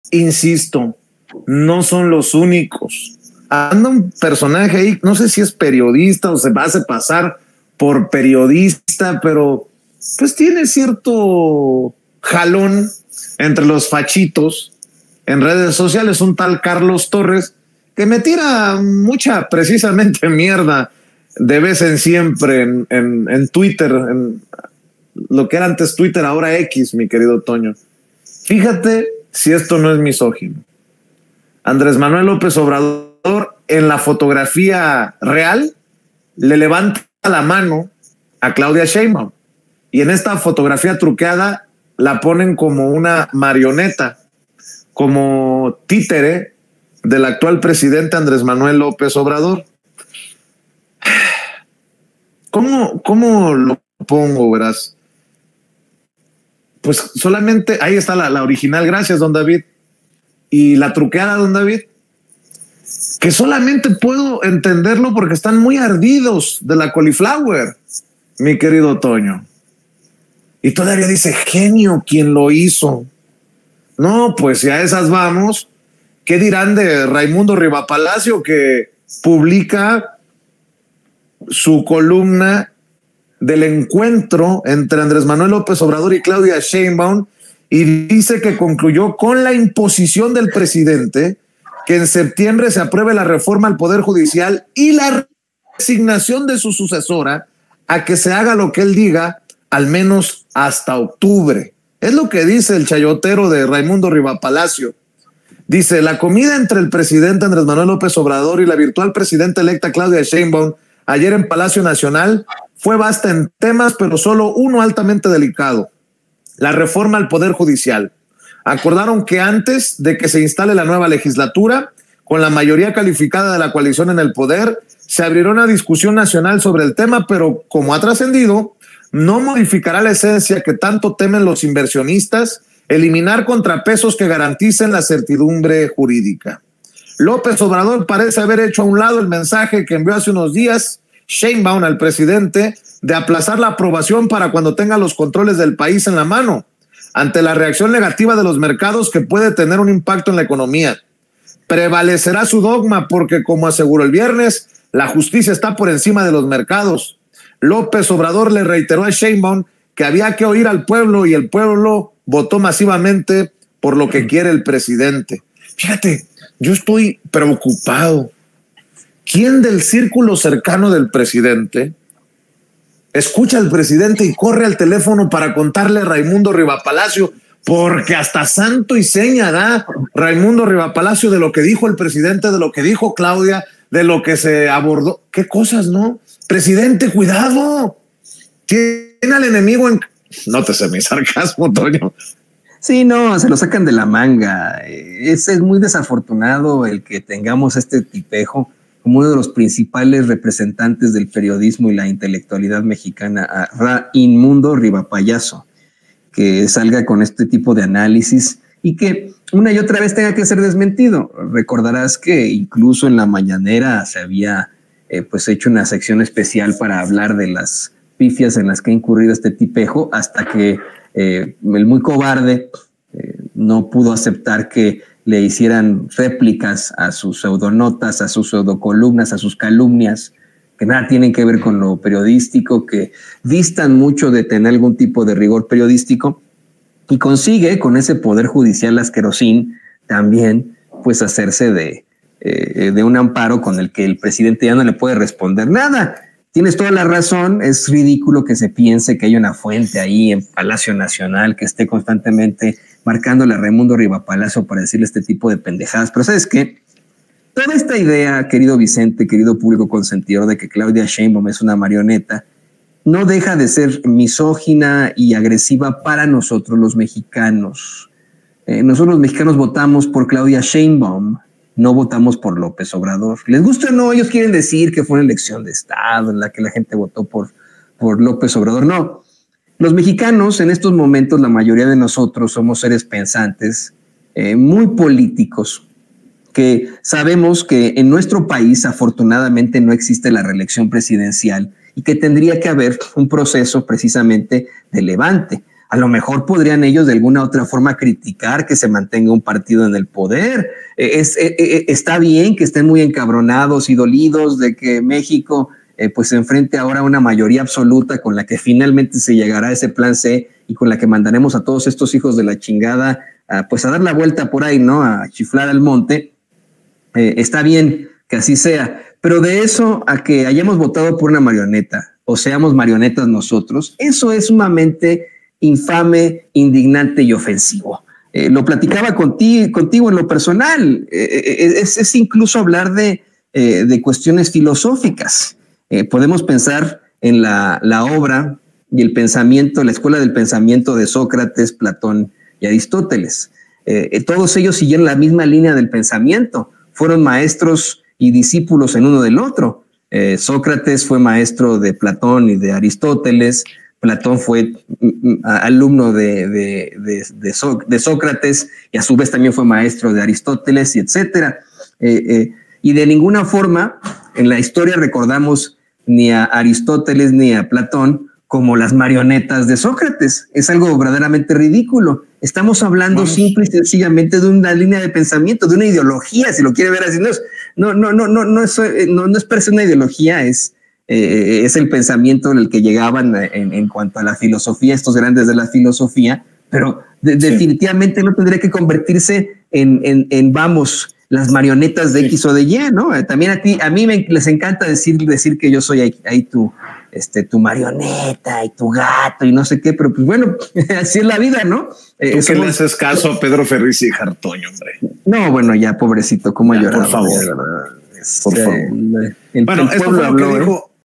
F4CH1TOS INMUNDOS EN DESESPERACIÓN ABSOLUTA INSISTEN EN DIFAMAR A SHEINBAUM COMO TÍTERE DE AMLO
Hay un tipejo de nombre Carlos Torres que tiene cierto jalón entre los fachitos, y que ahora compartió una foto trucada para poner a la Dra. Claudia Sheinbaum como una marioneta del presidente López Obrador. Por su parte, RaINMUNDO Riva Payaso publicó una columna también muy chafa en la que también sostiene una supuesta dependencia de la doctora al actual mandatario